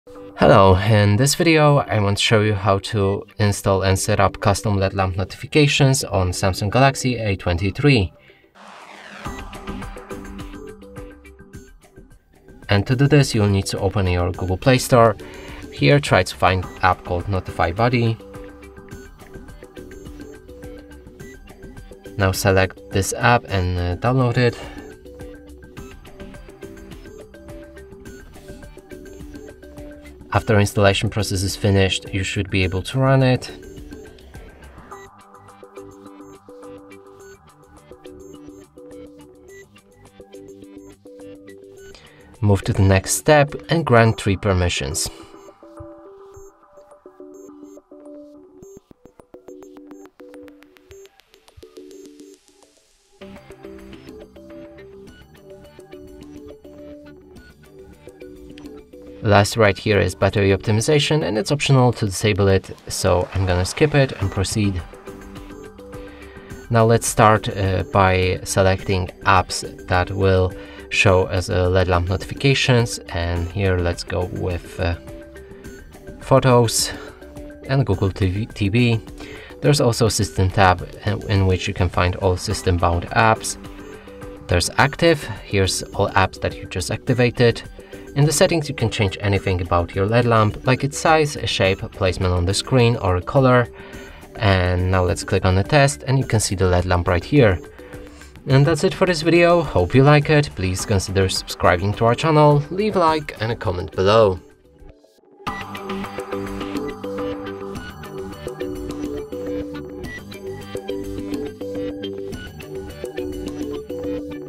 How to Set Up Custom Led Lamp Notifications on SAMSUNG Galaxy A23
Find out more info about SAMSUNG Galaxy A23:
Watch this tutorial if you want to set up custom LED lamp notifications on SAMSUNG Galaxy A23. We will show you how to install the NotifyBuddy app on your device, and then how to use it to customize LED lamp notifications on your Galaxy app.

How to set up custom LED lamp notifications on SAMSUNG Galaxy A23? How to change LED lamp notifications on SAMSUNG Galaxy A23?